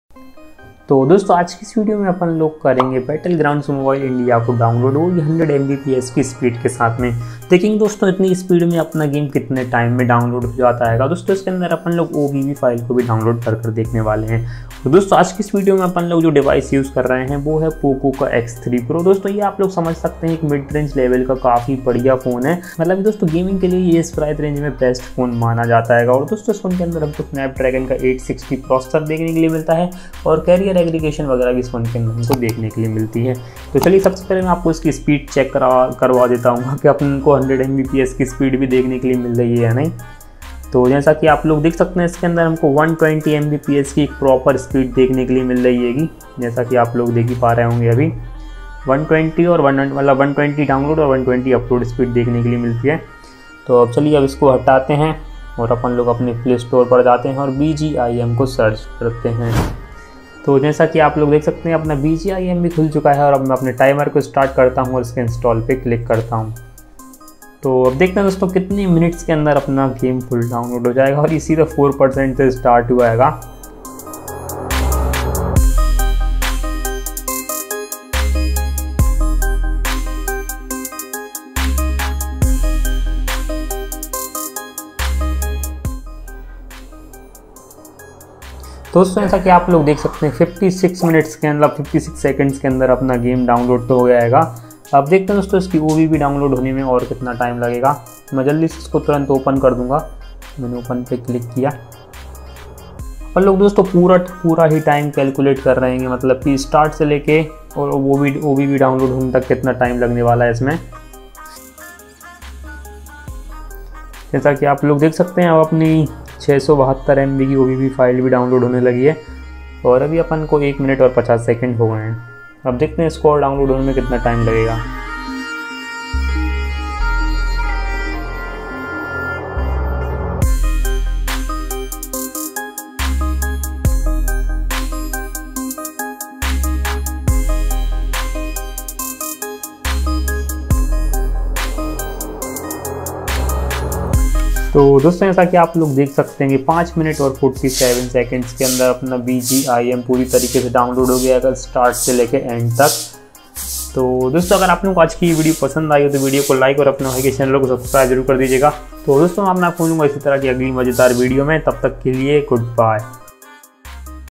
Downloading BGMI (Pubg) with 100MBPS Speed 🔥🔥 | Airtel Broadband Speed Test Pubg Download
Hello Friends In This Video I'm Downloading BGMI (PUBG Mobile) With Airtel 100MBPS Broadband Speed